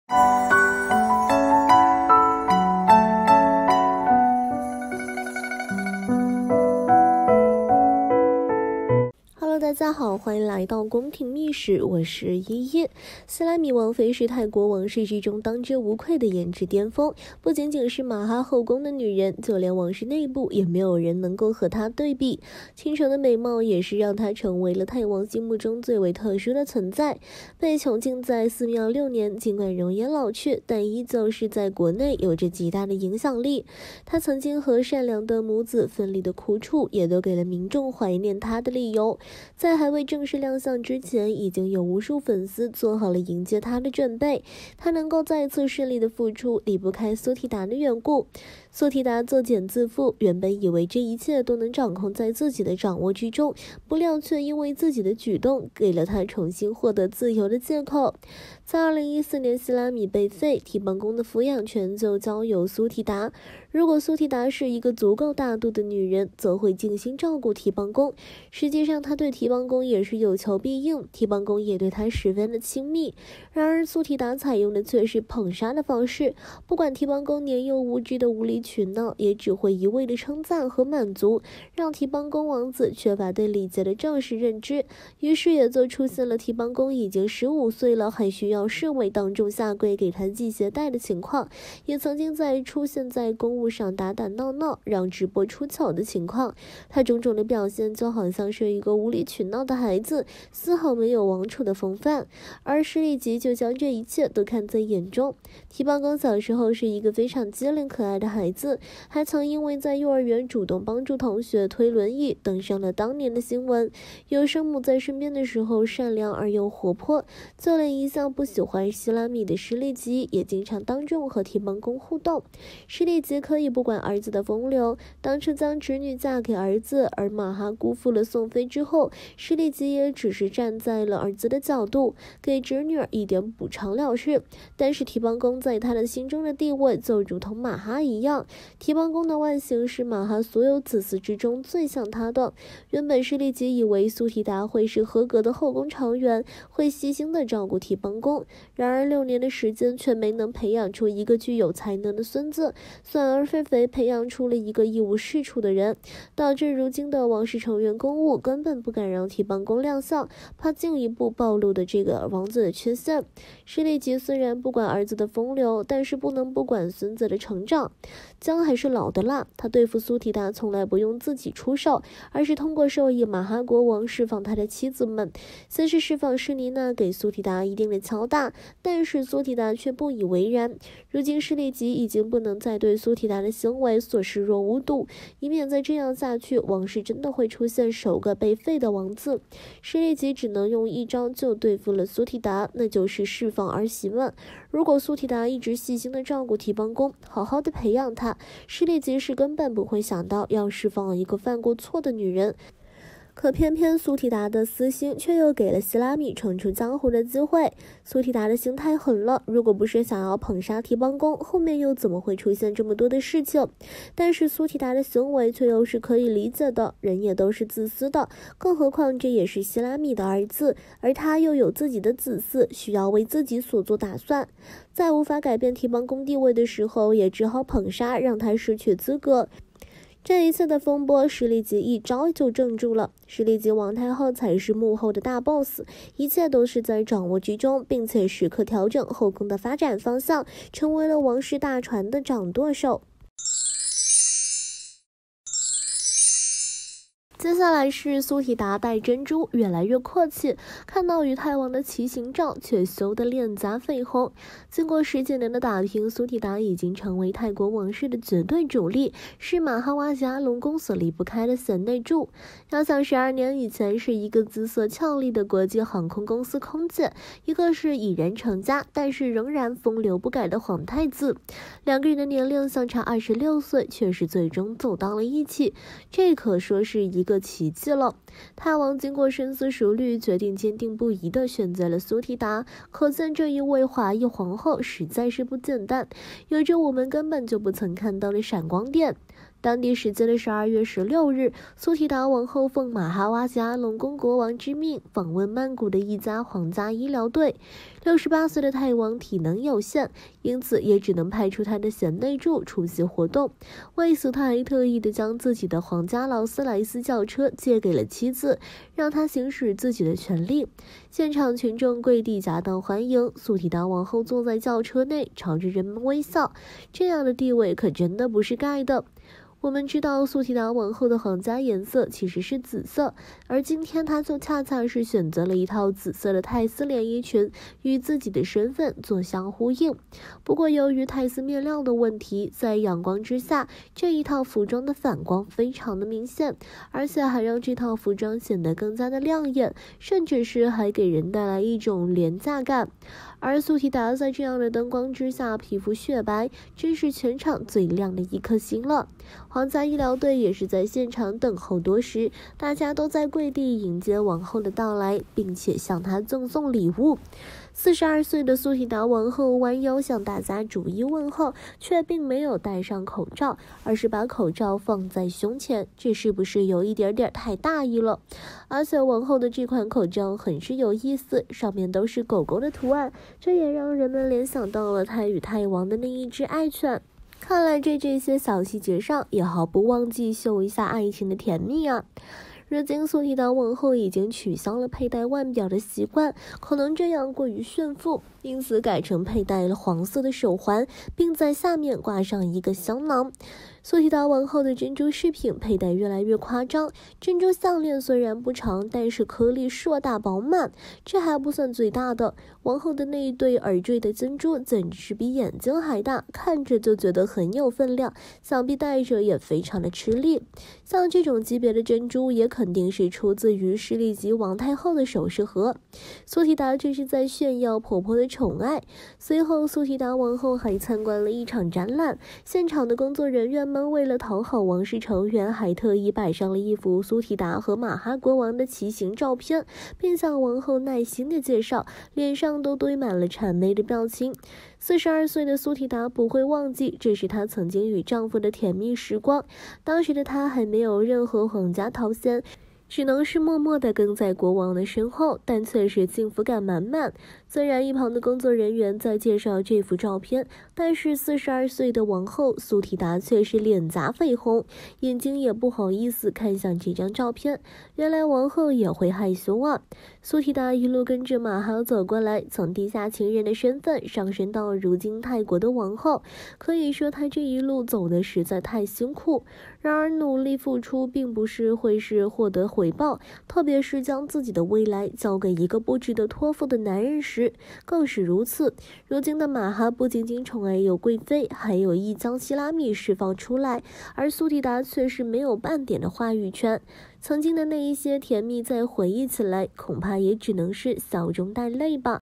蘇提達心太狠，故意「捧殺」提幫功惹怒詩麗吉，反而讓母子團聚｜宮廷秘史
蘇提達心太狠，故意「捧殺」提幫功惹怒詩麗吉，反而讓母子團聚